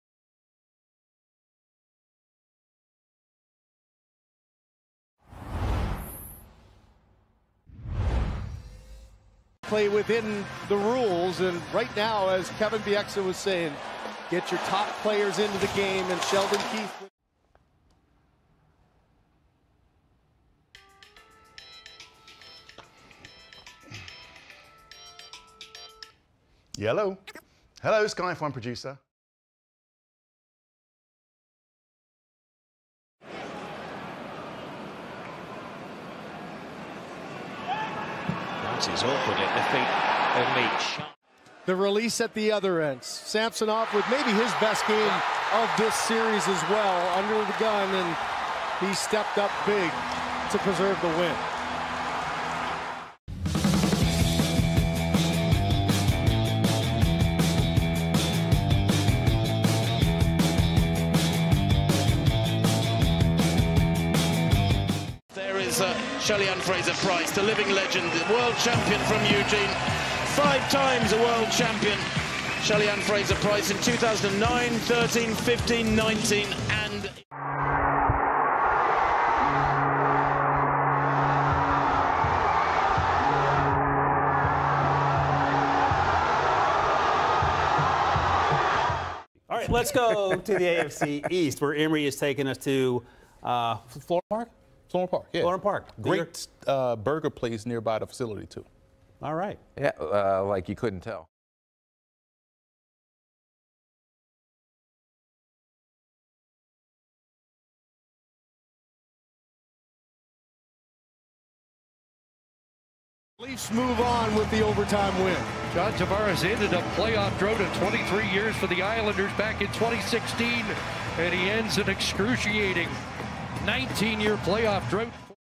What's Trending in Canada in Sports on Sunday, April 30th, 2023
9: Maple Leafs vs Tampa Bay Lightning Game 6 Post Game Analysis - April 29, 2023 | Game Over: Toronto